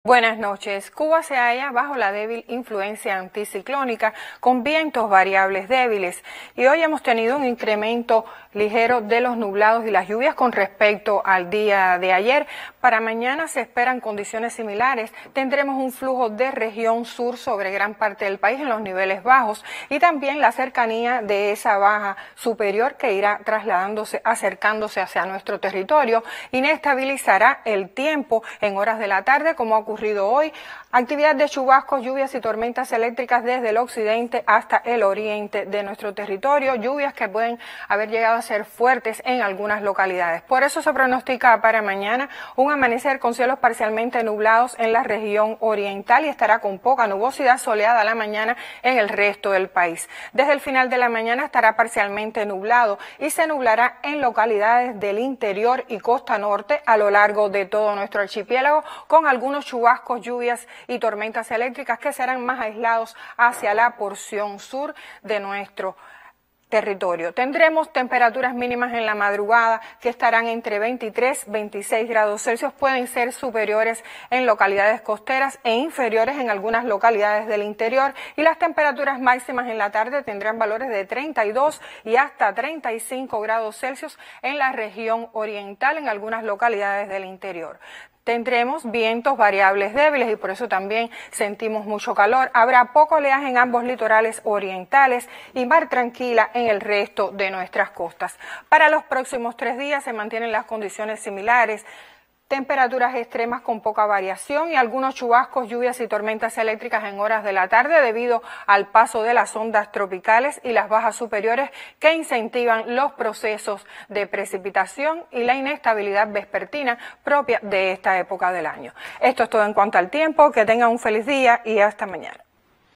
El Tiempo en Cuba
El Instituto de Meteorología (INSMET) de Cuba pronostica que desde el final de la mañana estará parcialmente nublado y se nublará en la tarde en zonas del interior y norte de gran parte del país con algunos chubascos, lluvias y tormentas eléctricas… Conozca cuáles son las principales variables meteorológicas para esta noche y los próximos días en la emisión Estelar del NTV